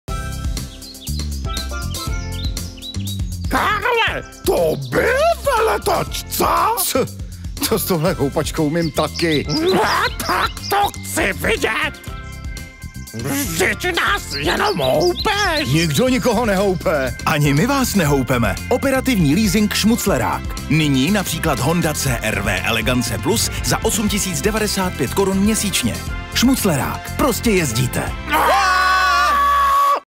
Honda CR-V Elegance Plus | Šmuclerák
Koukáte už delší dobu po SUV? Co takhle zkusit oblíbenou Hondu CR-V? S operativním leasingem od Autocentrum Jan Šmucler může být vaše. Se šmuclerákem nemáte žádné starosti, prostě jezdíte... V nabídce máme i další vozy značek Škoda, Seat, Honda a Volkswagen. Podívejte se na a vyberte si.

Autocentrum Jan Šmucler